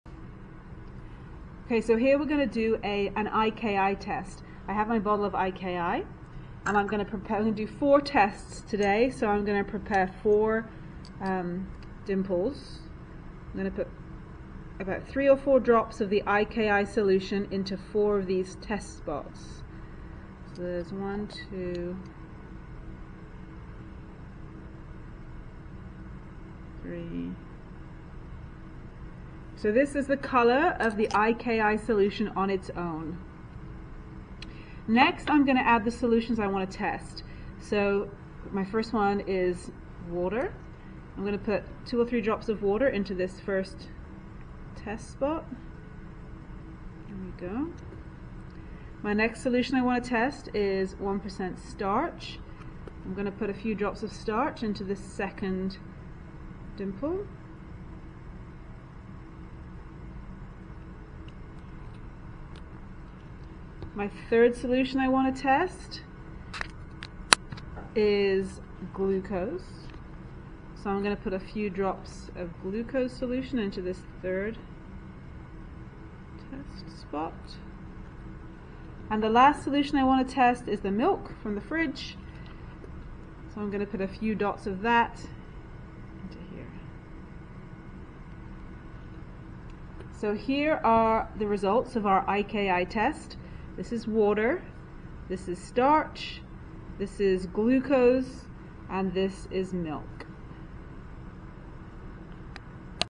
IKI test
This video shows an IKI test on water and solutions of glucose, starch and milk